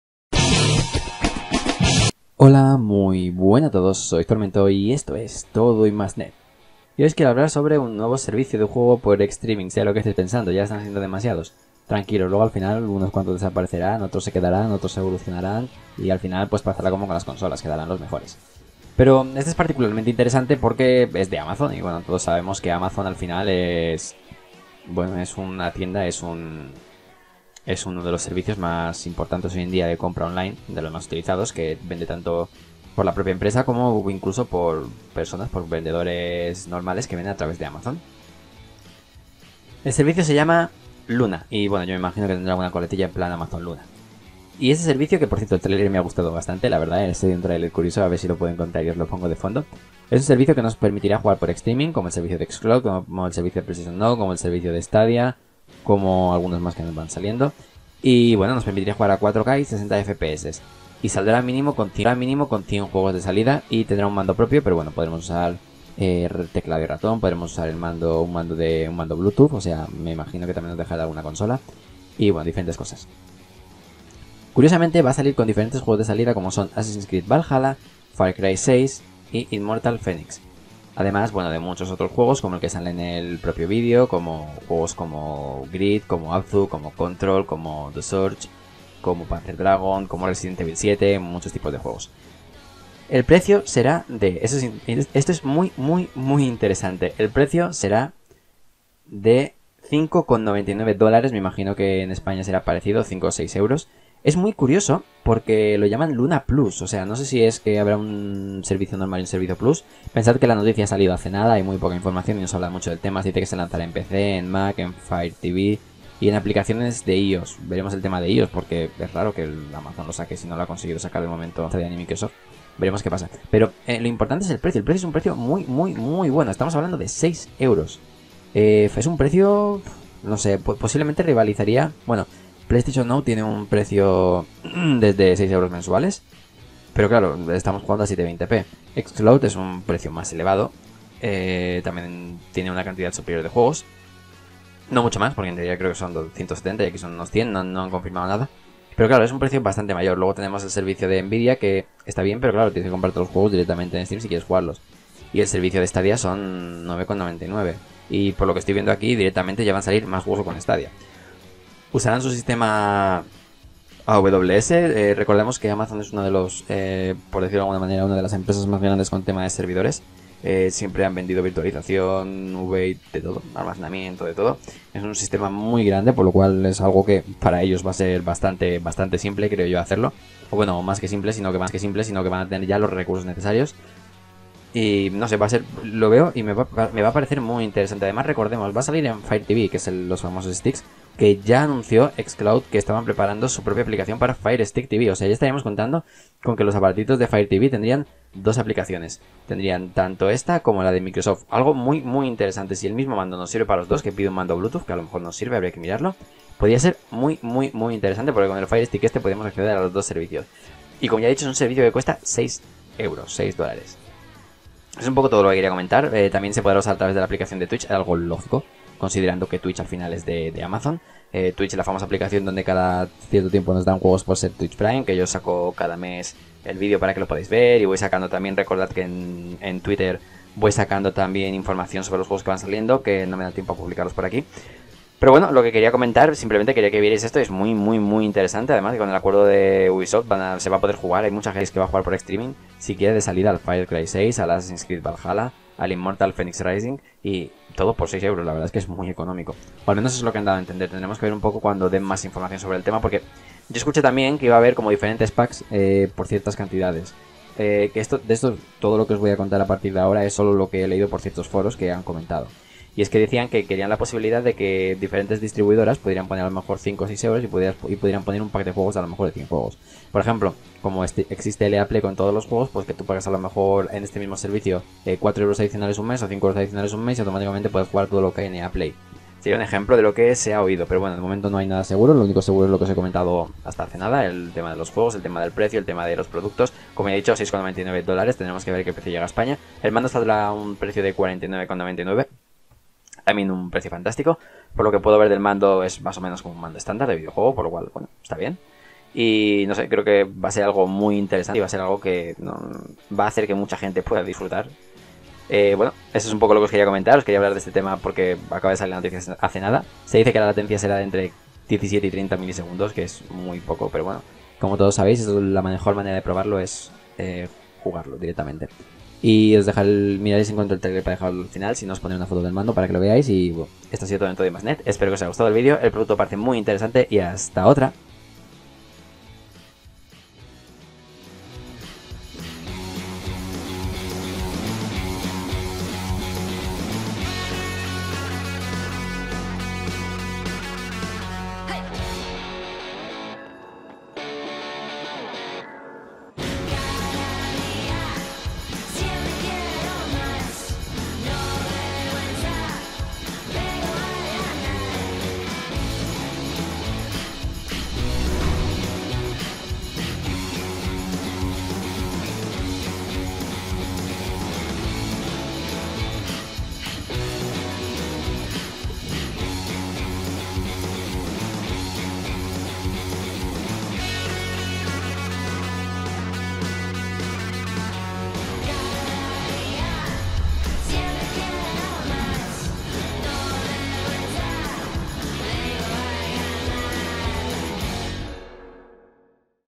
[AMAZON LUNA] el servicio de streaming de videojuegos por solo 5,99$
Amazon ha hecho oficial Amazon Luna,  un servicio de juego por streaming.

👇si queréis que YouTube os avise cuando publiquemos un vídeo💗